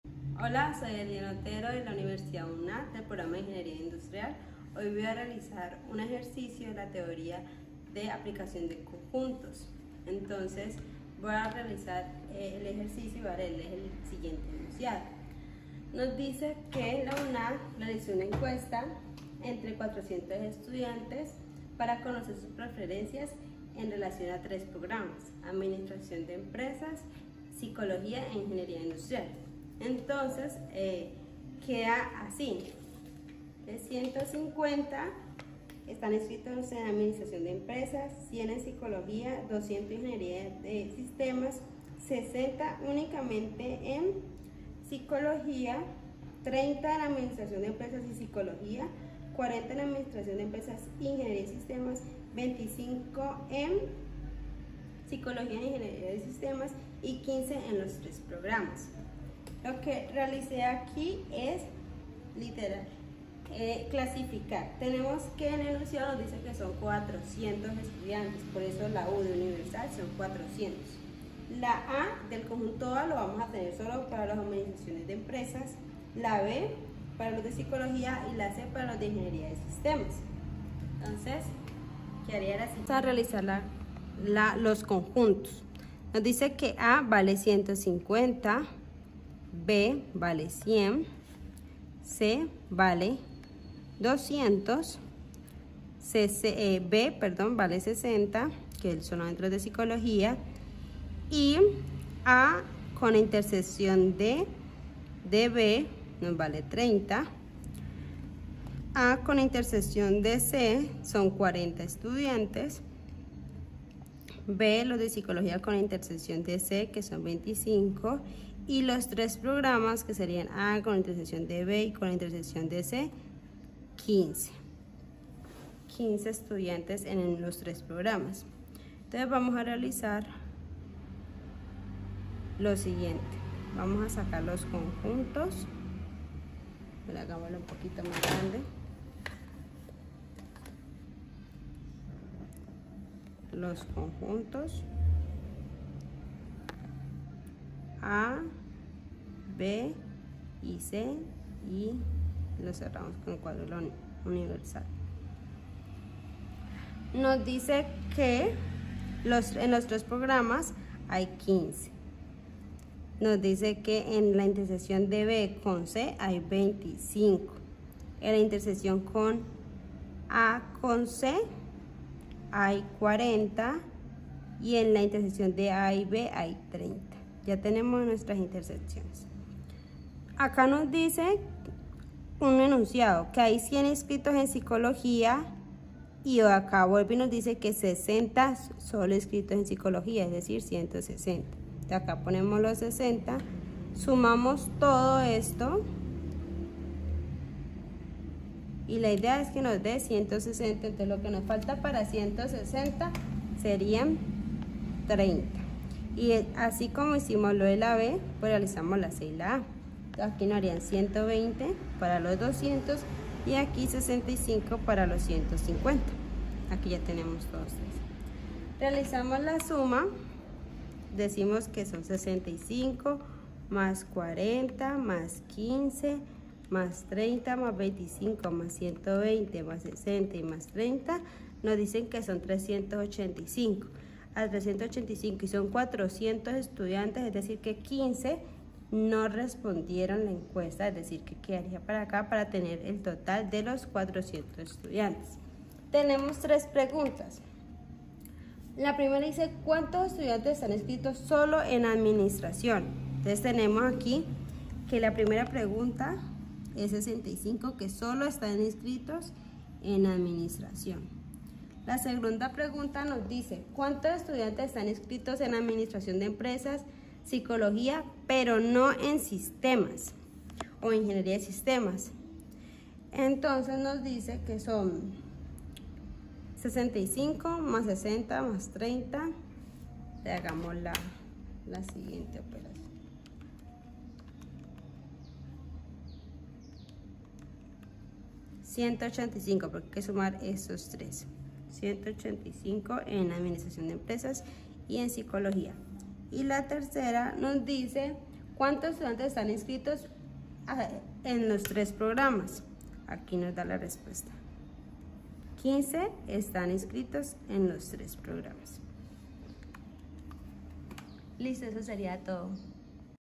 EJERCICIO 3 APLICACIÓN DE TEORIA DE CONJUNTOS -UNAD
Ejercicio 3 con letra E 2024, video explicativo. de la universidad UNAD